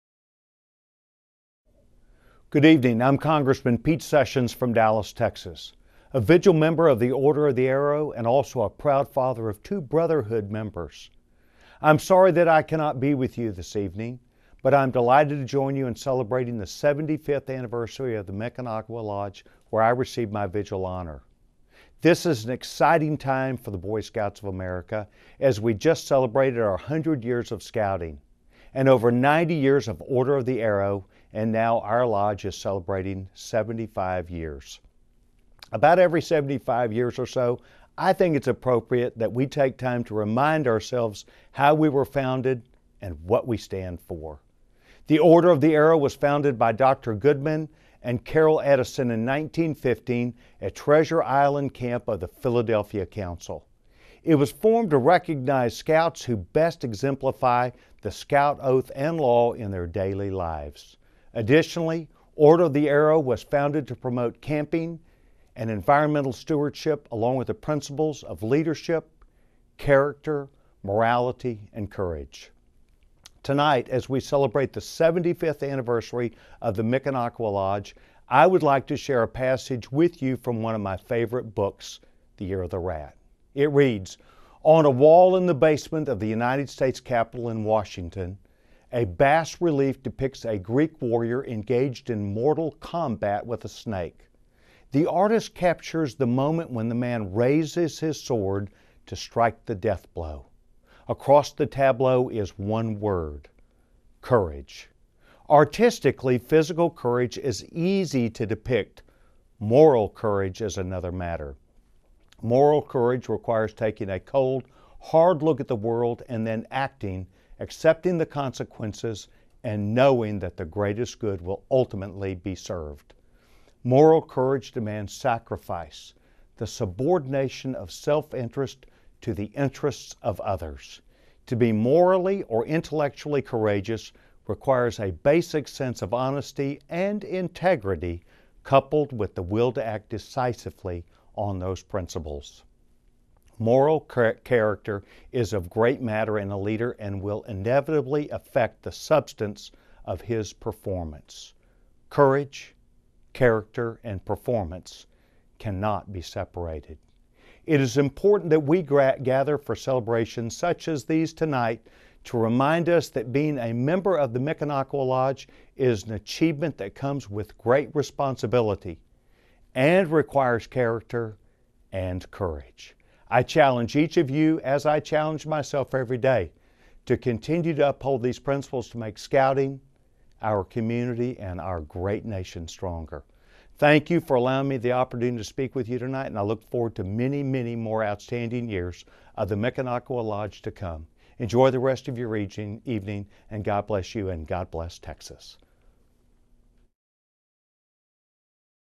Congressman Sessions Congratulates the Mikanakawa Lodge on its 75th Anniversary
A proud Eagle Scout and Vigil Member of the Order of the Arrow, Congressman Sessions congratulates the Mikanakawa Lodge of Dallas on its 75th anniversary.